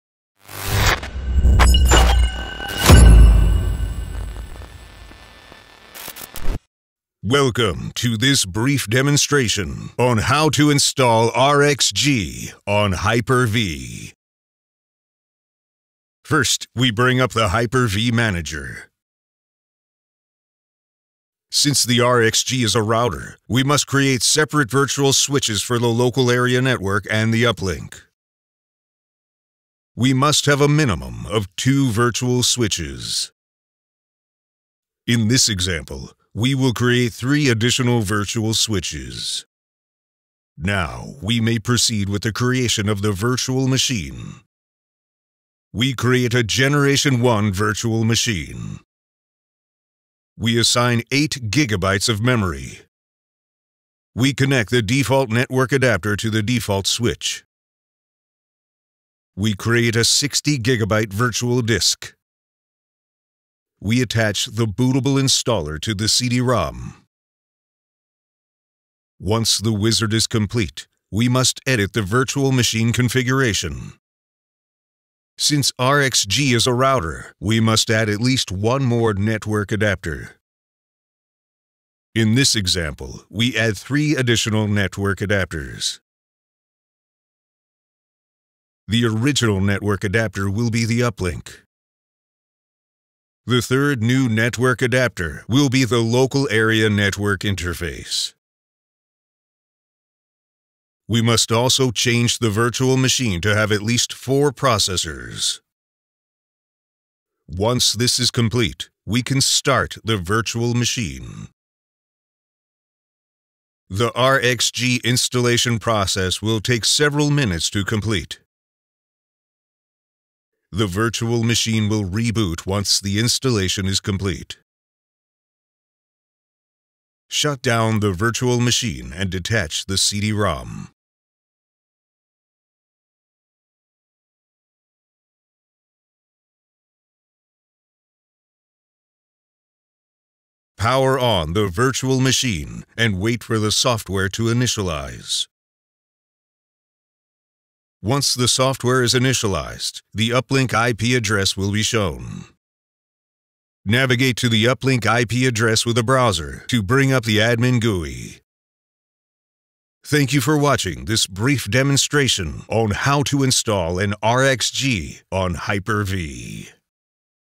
rXg Hyper-V Installation
This basic setup guide shows you how to install the rXg on Hyper-V. This video assumes that you already have Hyper-V running. Microsoft Hyper-V is available on 64 bit versions of Pro and Enterprise Windows 8, 8.1 and 10. It is also available on Windows Server 2008 and later.